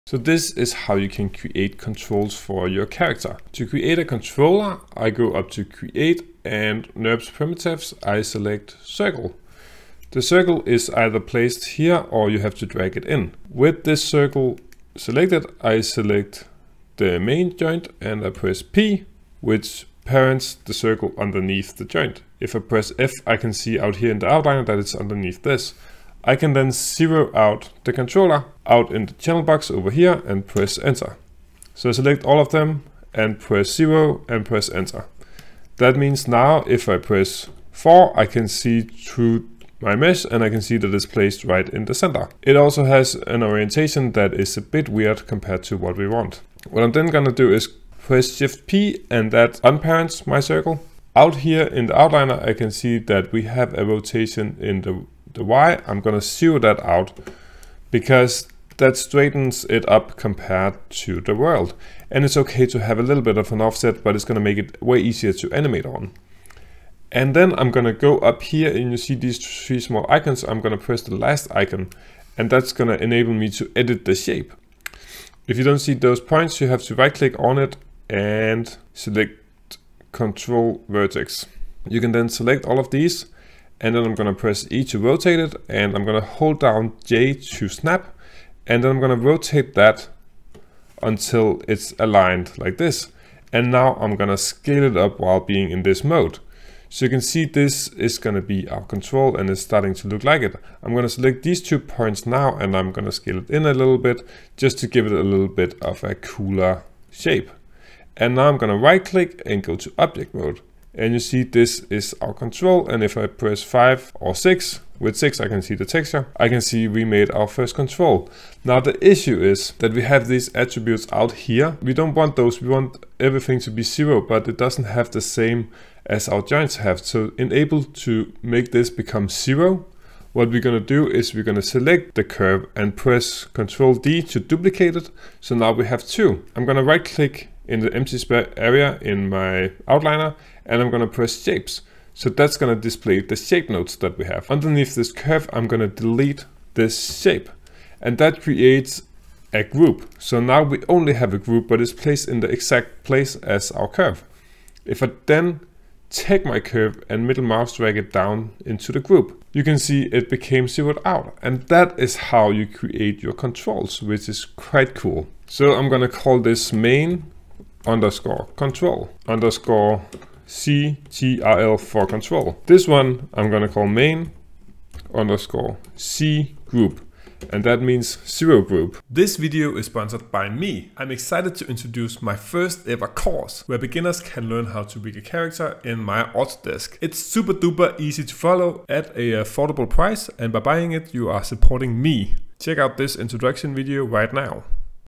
How To Make A Controller In Maya
In this video we cover how to create a rig controller for a character in Maya Autodesk. We do so in a fast and easy to follow tutorial. You can get my full course as well on FlippedNormals where we cover everything in regards to rigging a character in Maya.